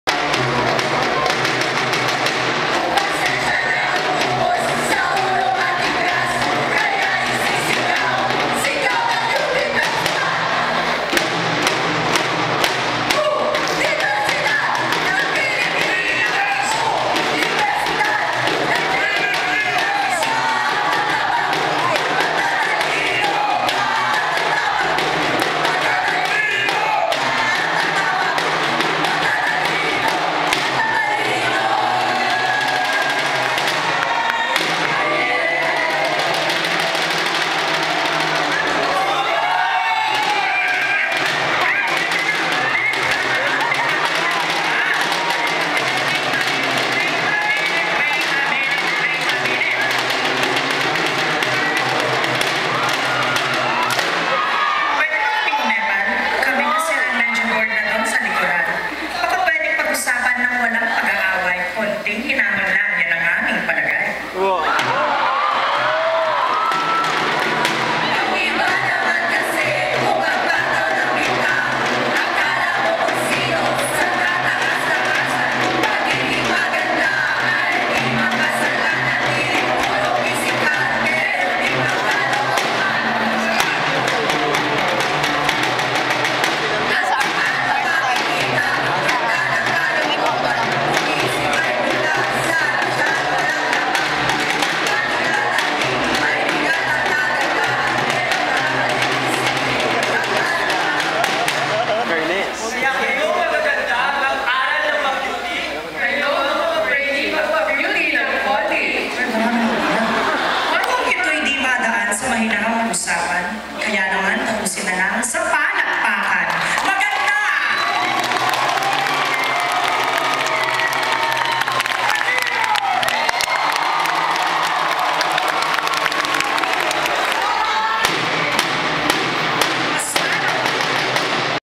U.P. Diliman Faculty Follies 2008 Featuring National Artists Virgilio Almario & Bienvenido Lumbera
Hagikgikan 2008
University of the Philippines Faculty Follies
University of the Philippines Theater
15 February 2008
3:00 and 7:00 p.m.

U.P. College of Arts and Letters features National Artists Virgilio Almario (as Ugly Betty) and Bienvenido Lumbera (as Marian Rivera / Rufa Mae Quinto) in a stylized Balagtasan number (Malakas vs. Maganda)